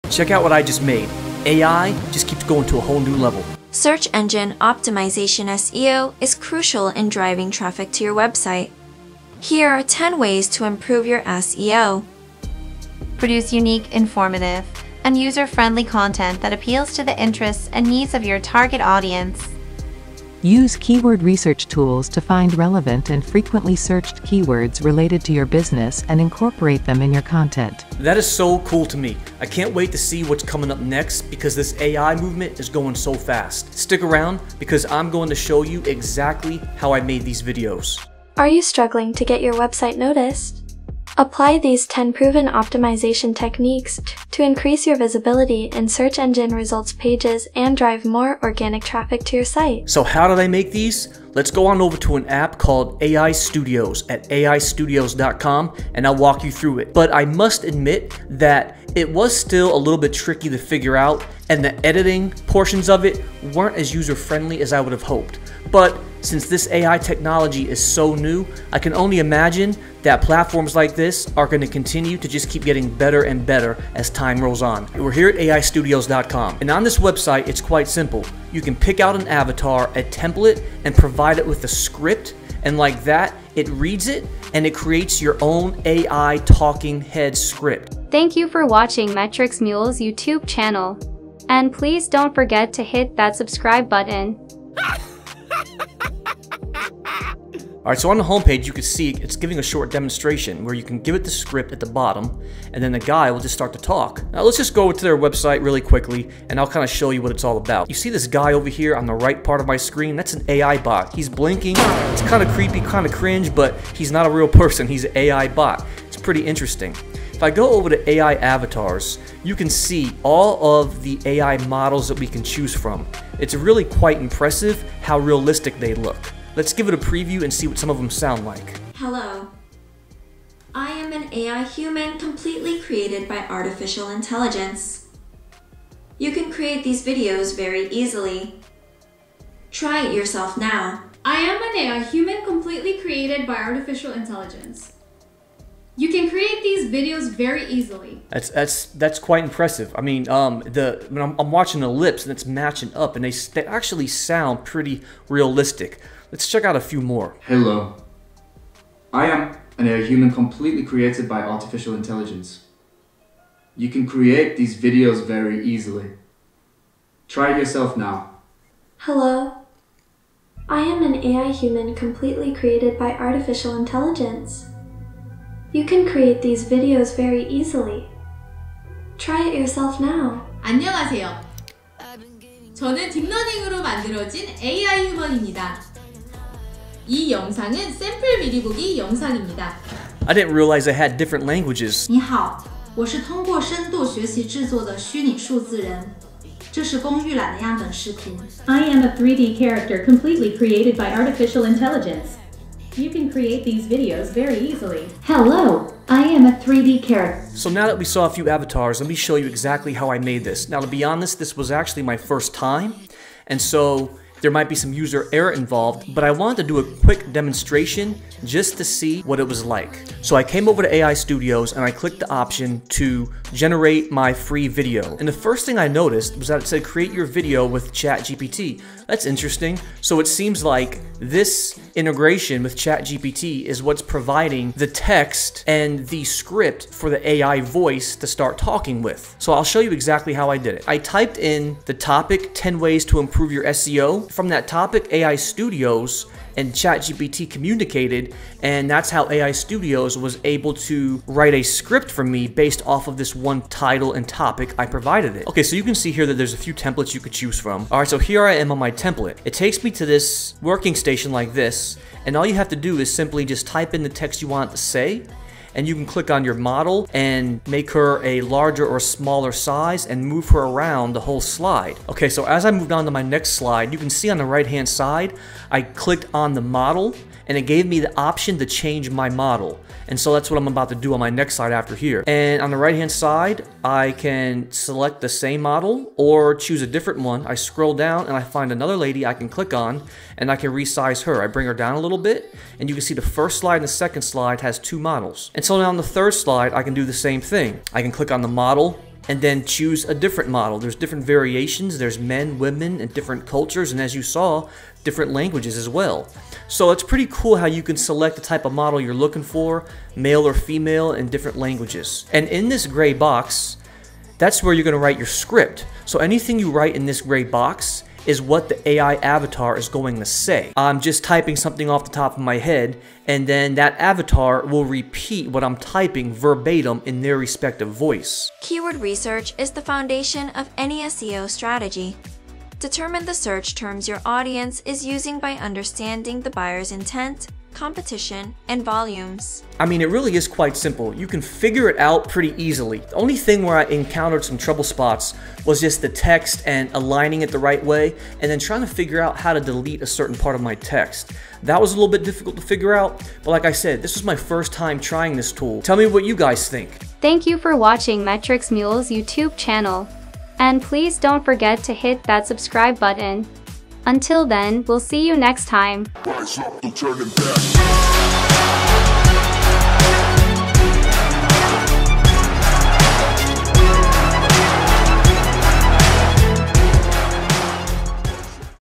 Generate Your Own AI News Anchors
With this app, you can create realistic avatars with the help of ChatGPT.

This AI Video Generator is called AI Studios, and it integrates with ChatGPT.

Provide the AI Video Generator with a topic (it uses ChatGPT AI), and the AI Avatar will create a video presentation with the data ChatGPT fed it.

It's amazing.

If you're familiar with ChatGPT, then you know that you type in a prompt of what you want and ChatGPT responds back to you via text. This works the same way. But instead of responding via text, the AI Avatar responds to you in realistic verbal language.

I'll show you how I did it.

Camera I use for filming
Elgato Key Light